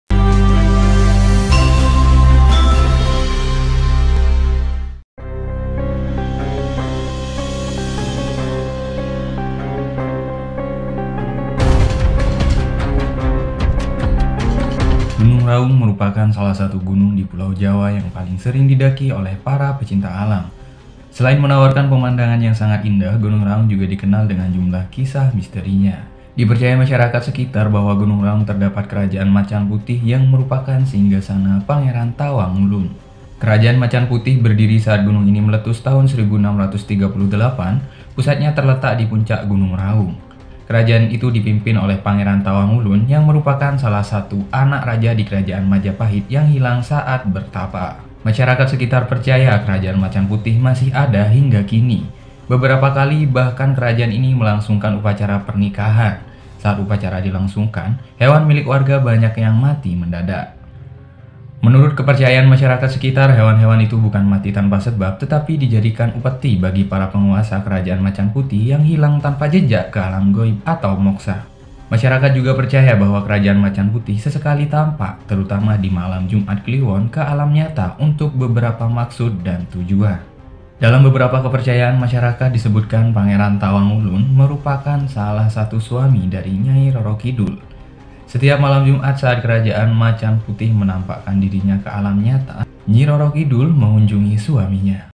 MISTERI GUNUNG RAUNG
Gunung yang ada di jawa timur ini memiliki keindahan yang menarik banyak pecinta alam, namun taukah kamu gunung raung juga menyimpan banyak sekali misteri